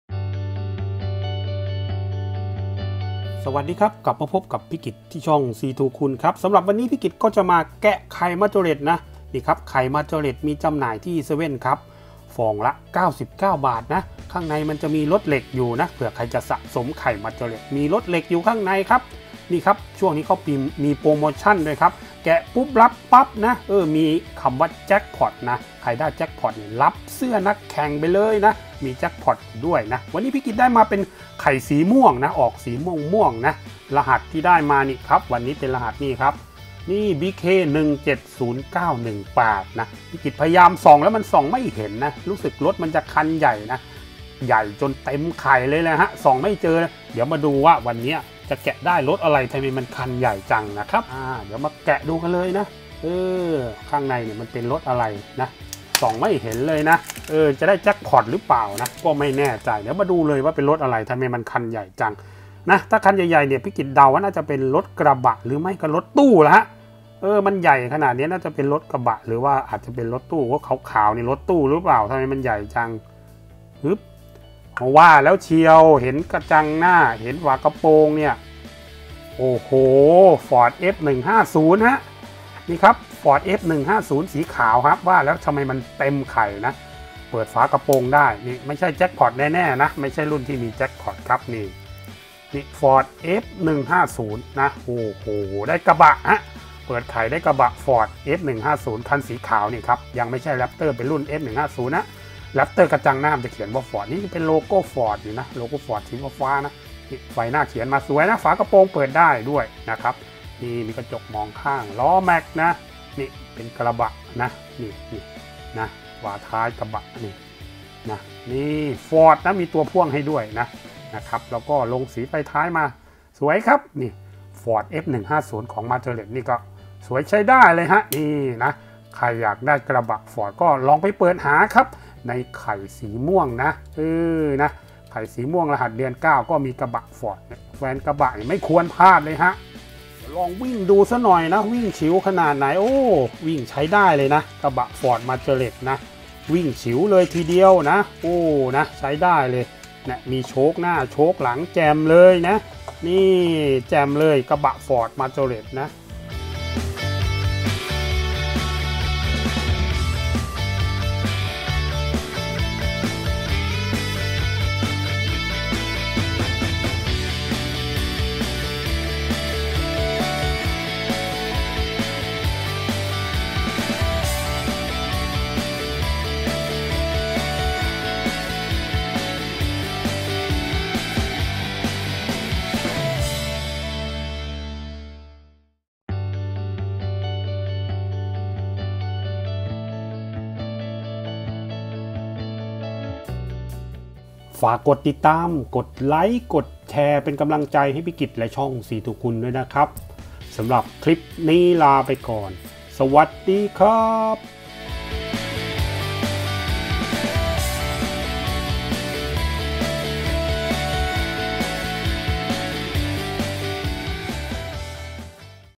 🥚แกะไข่ได้กระบะ🚚ไข่รถเหล็กมาจอเร็ต|C2Kun
ลุ้นไข่แจ็คพ็อตได้กระบะคันสวย ฝากกดติดตามเป็นกำลังใจด้วยนะครับ
ติดตามชมคลิปใหม่ช่อง C2Kun ได้ทุกวันที่นี่
add line เพื่อเข้ากลุ่มรถเหล็ก มาจอเร็ตนะครับ Line ID = moskit2002